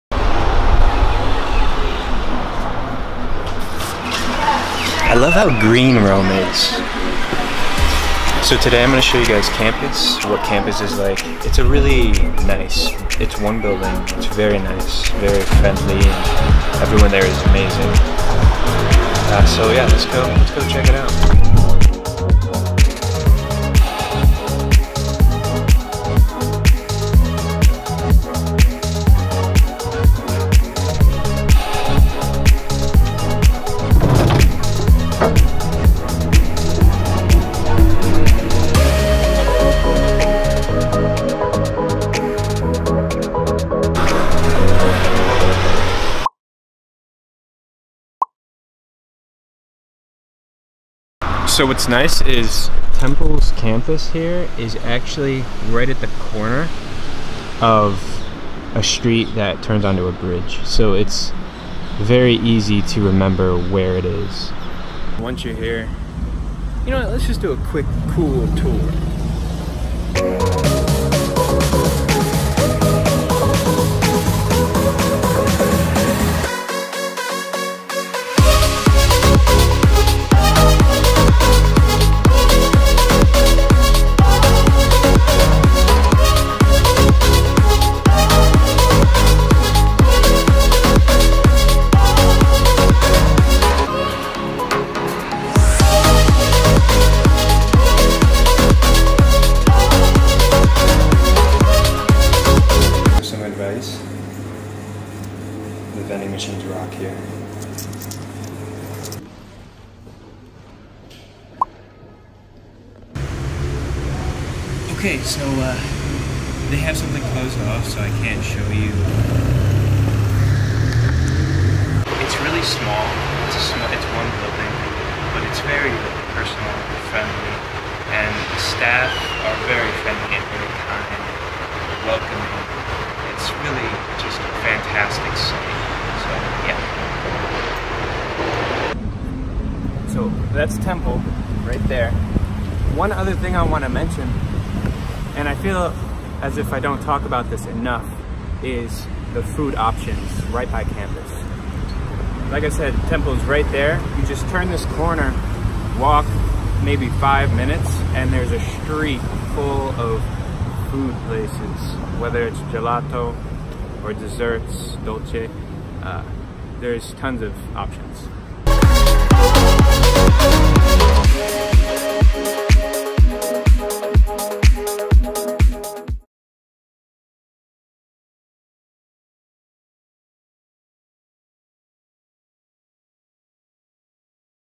Jordan Allison Episode 4 - Temple's Rome Campus and Caffè
In this episode, Jordan takes us on a tour of Temple University's Rome Campus. Check it out!